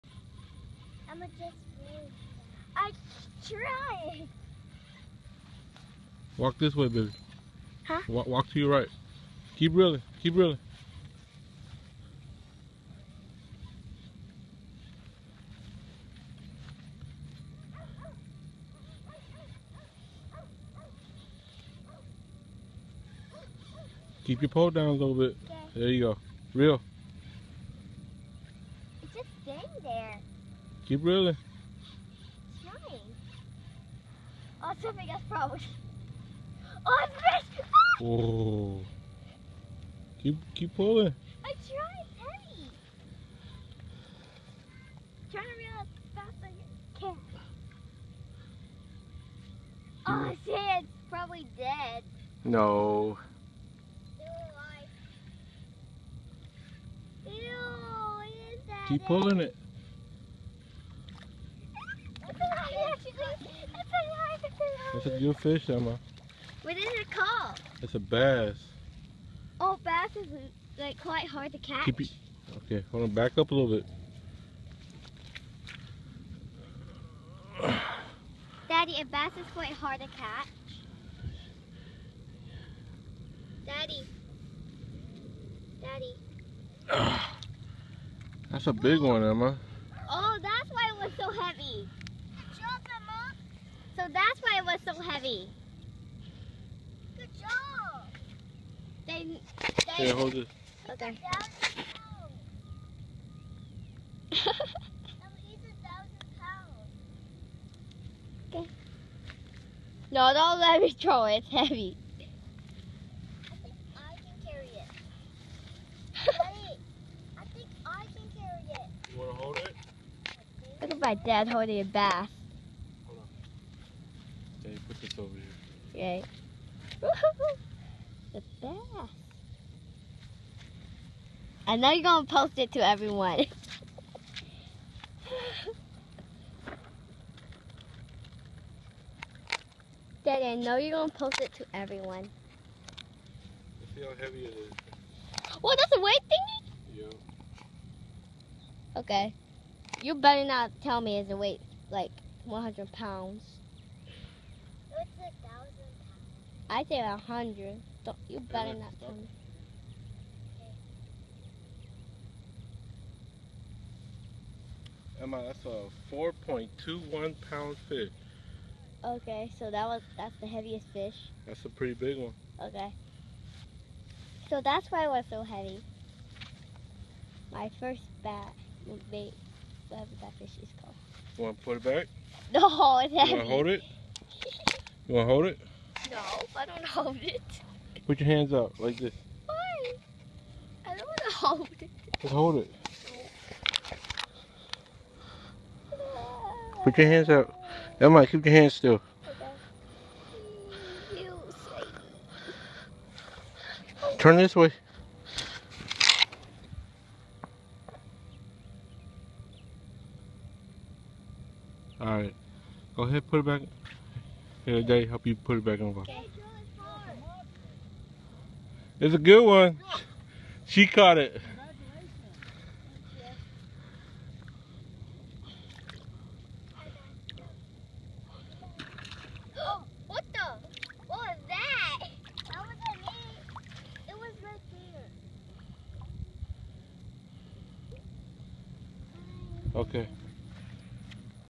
Huge Bass Slams Emma rod
Breaking in her Billystix custom rod she just got for her birthday nicely ! 4.21 lb bass